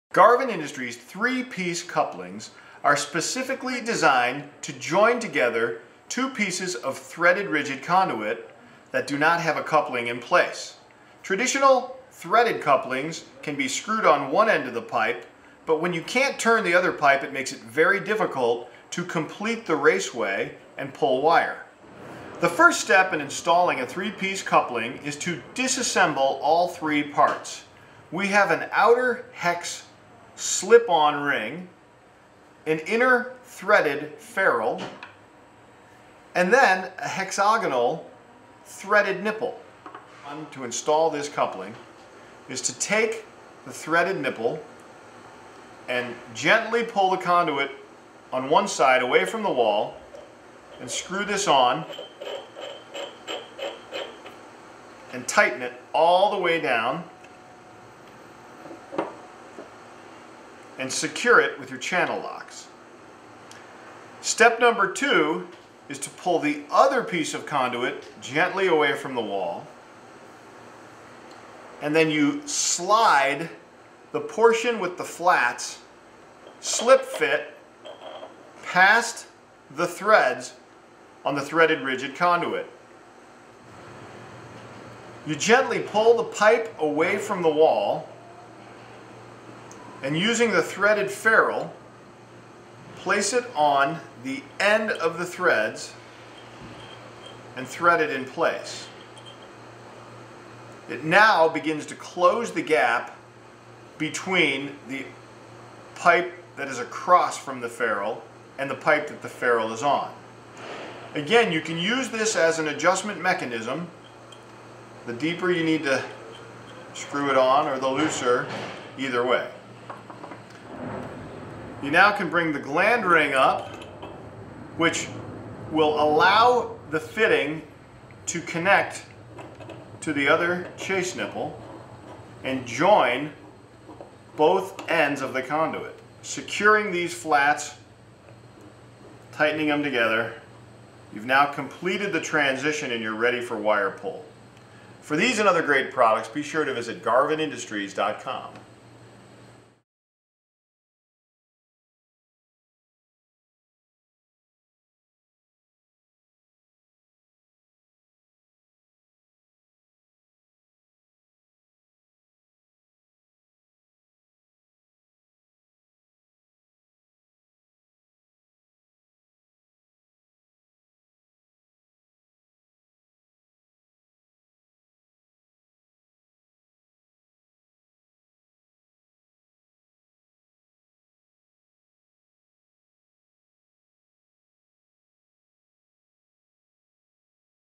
5" Zinc Plated Malleable Iron Three Piece Coupling: 3PC500
UL listed 5" three piece couplings are used to connect 5" threaded rigid or IMC conduits that are not able to be turned. Garvin Industries' three piece couplings provide concrete tight connections. Garvin Industries' heavy gauge construction maintains mechanical protection and solid grounding of one conduit to another. Three piece couplings are zinc electroplated for excellent corrosion protection in damp locations.